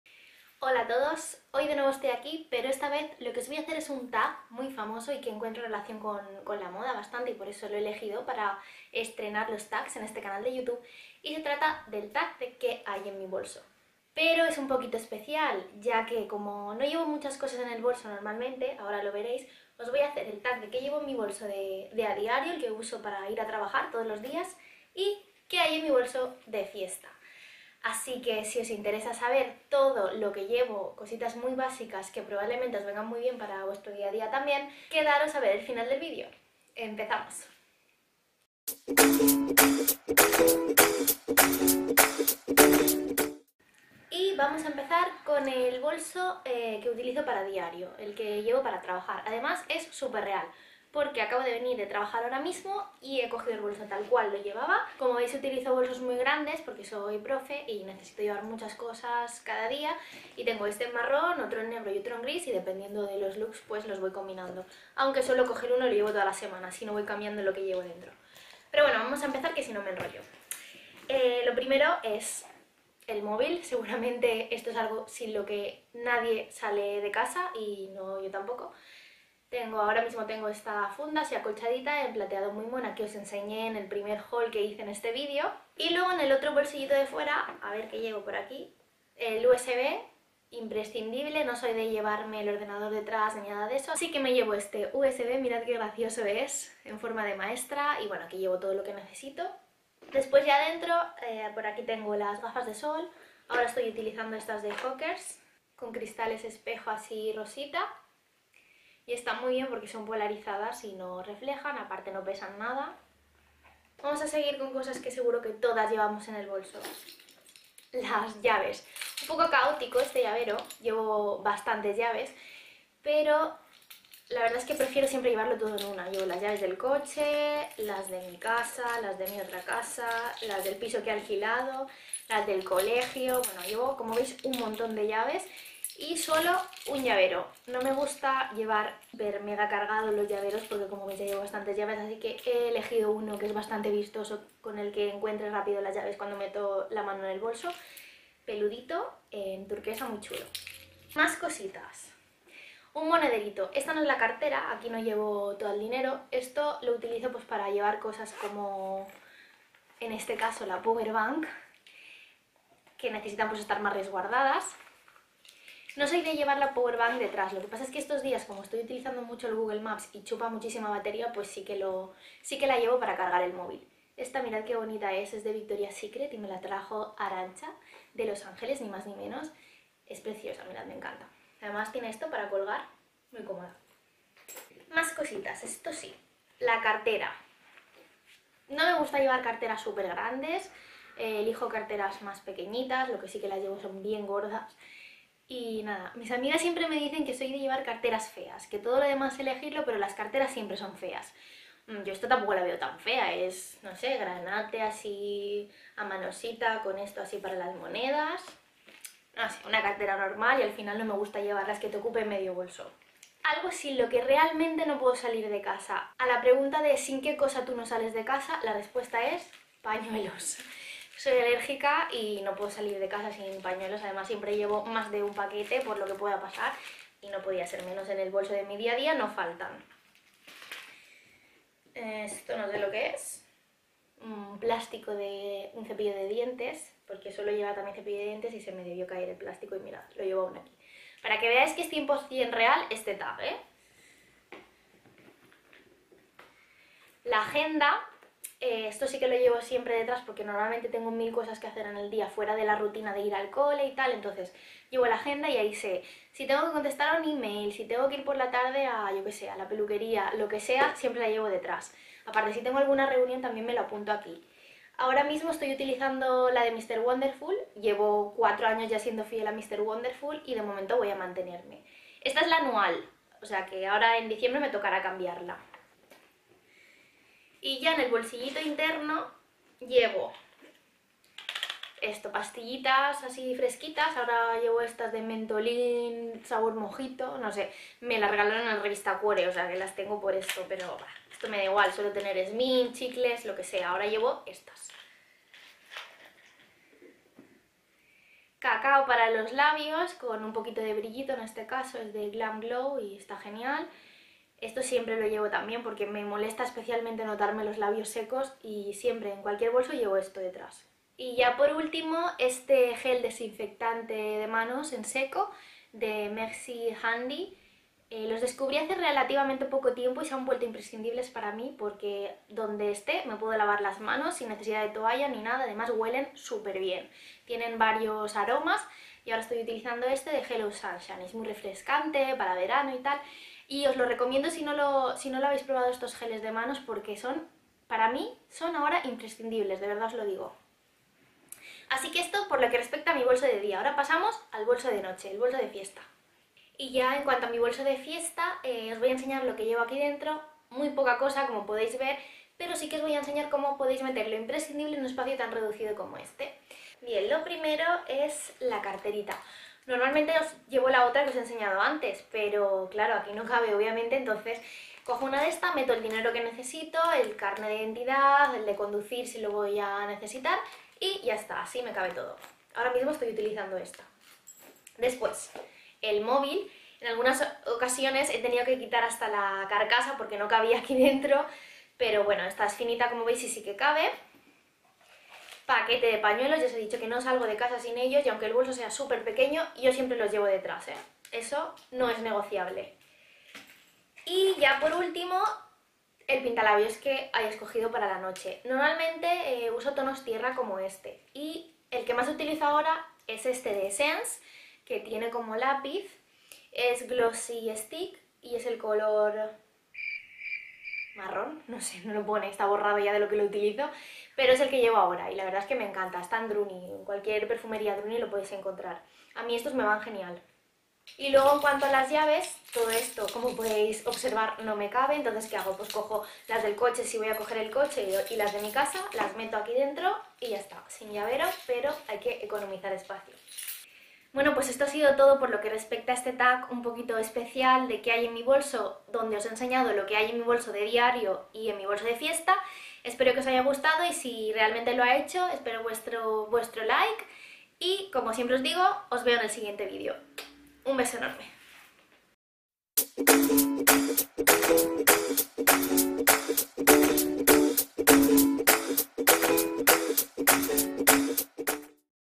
Tag ¿Qué hay en mi bolso? De diario y de fiesta ;)
En este famoso Tag ¿Qué hay en mi bolso?, os muestro lo que llevo a diario para ir a trabajar, pero también todo lo que cabe en mi mini bolsito de fiesta. ¡Descubre cuáles son mis imprescindibles y cuéntame si coincidimos!